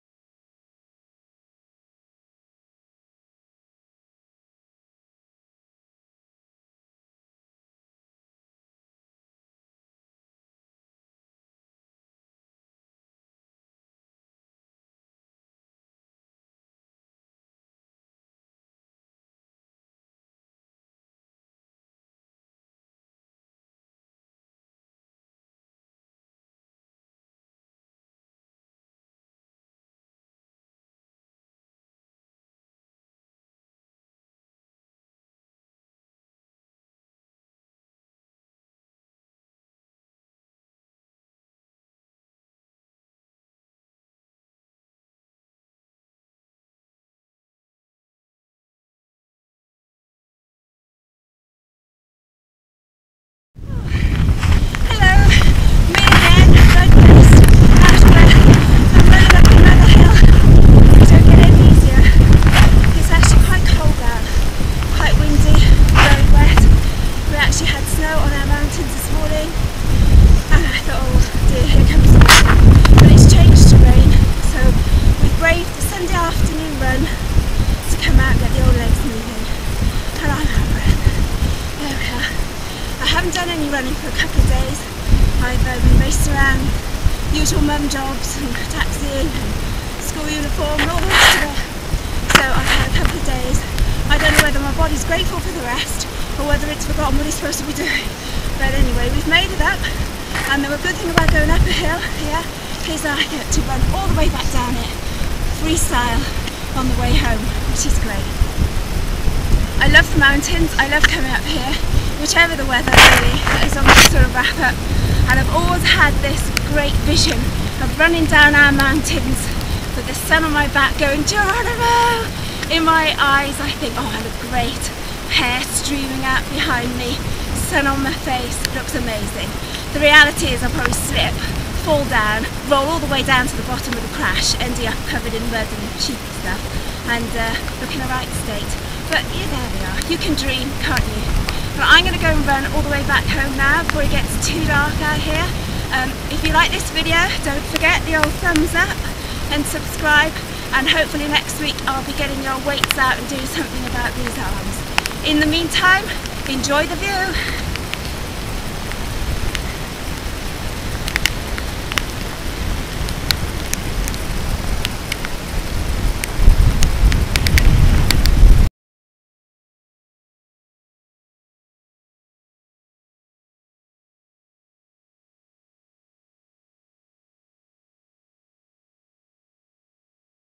MY ENERGETIC PLANTBASED LIFE 4 WET, WINDY AND RUNNING
TRAINING FOR A HALF MARATHON AND TRYING TO CHANGE MY LIFE AND BODY ON A PLANTBASED/RAWVEGAN DIET